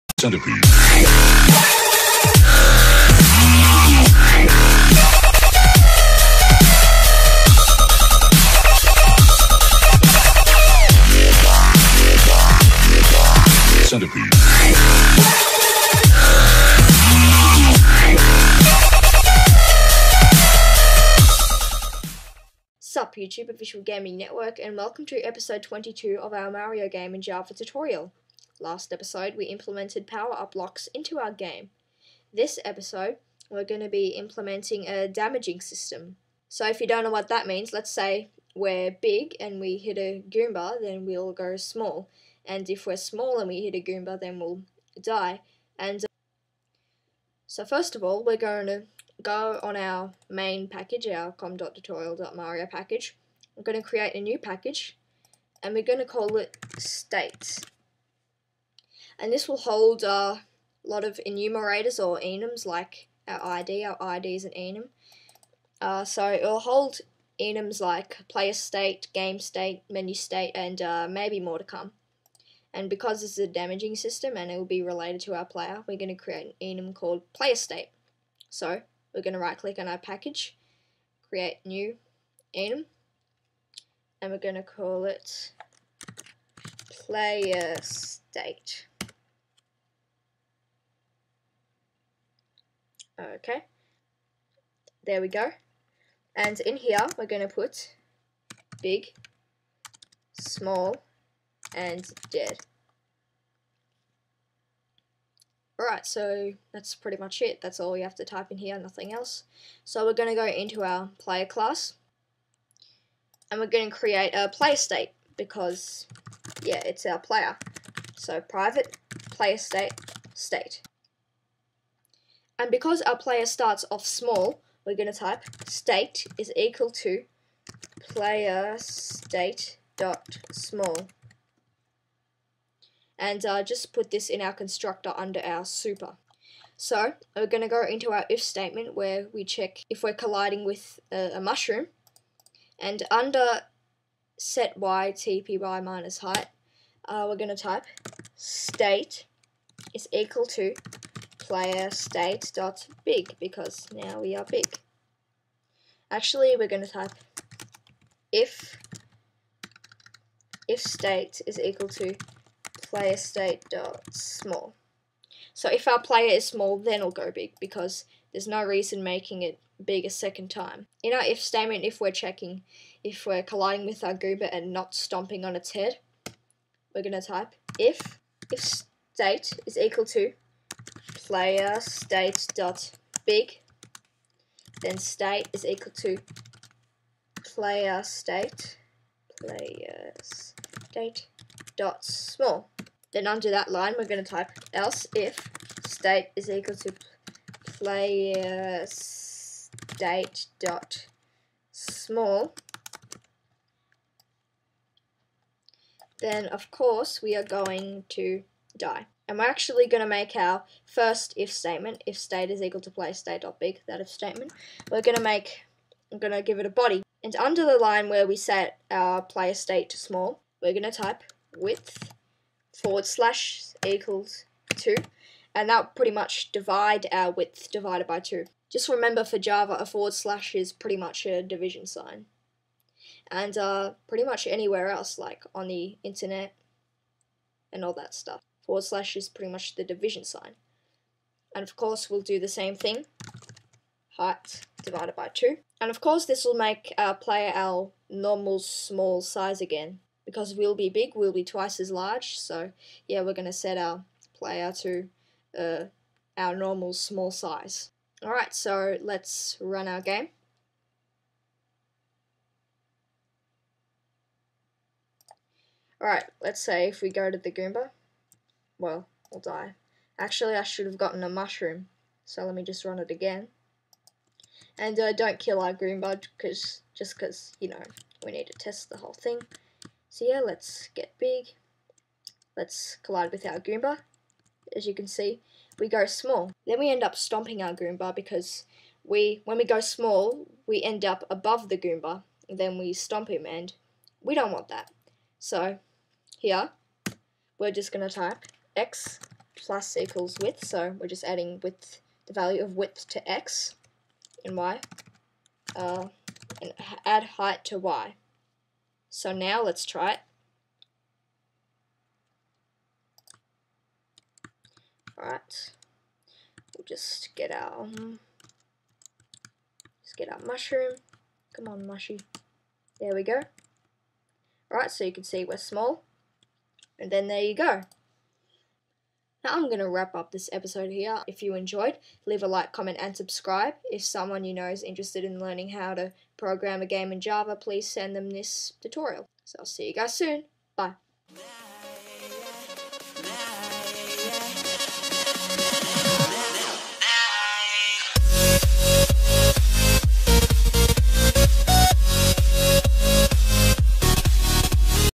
"Java Programming Tutorial" | "Super Mario Game" #22 | Damaging System
Welcome to episode 22 of our mario game in java tutorial! This episode, we implement a damaging system into our game.